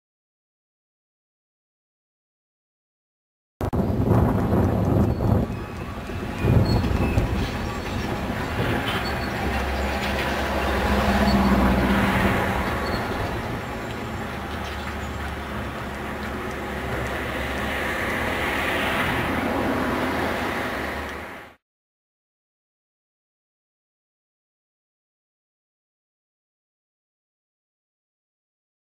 Desserte de l'usine Arcélor
...a Uckange
Rame de wagon a coke vide tracté par une G 1000 Vossloh